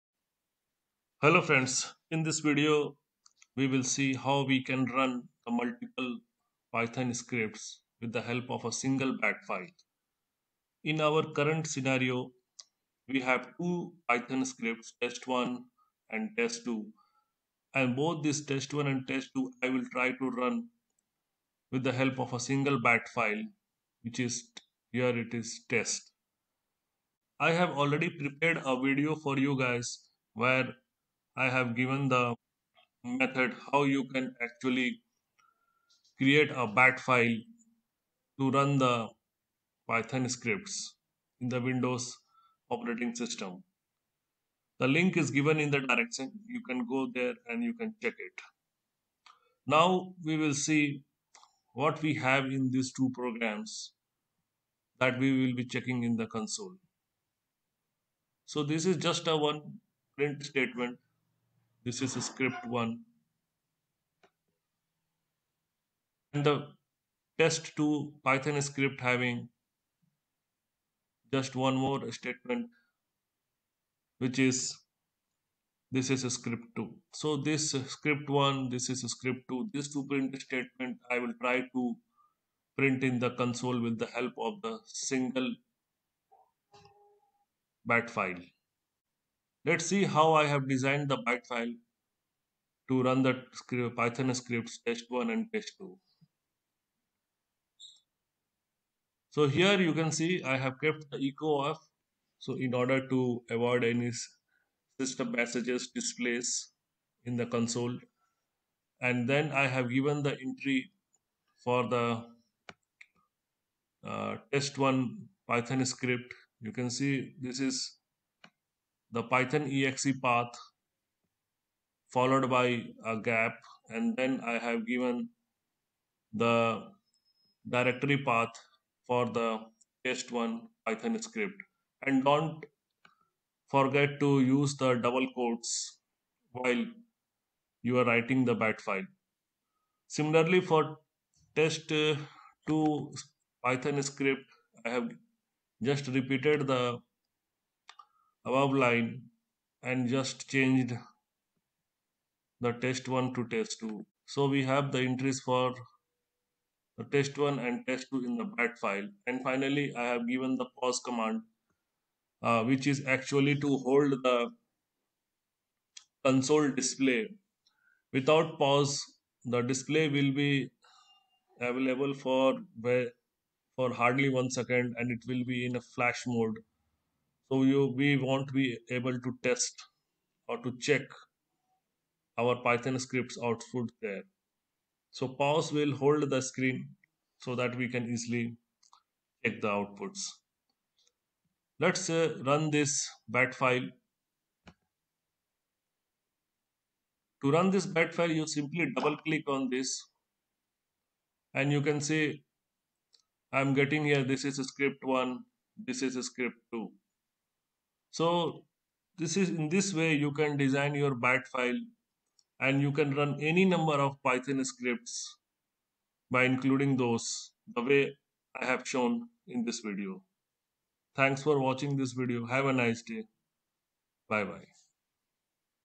How To Run Multiple Python Scripts with the help of Single Batch (.BAT) file
Link - How to run the python script from a BAT file and how to create the bat file.

Link- Python Path variable entry in environment variable. How to fix 'python' is not recognized as an internal or external command.:

Link: How to fix Python is not recognized as an internal or external command in Pycharm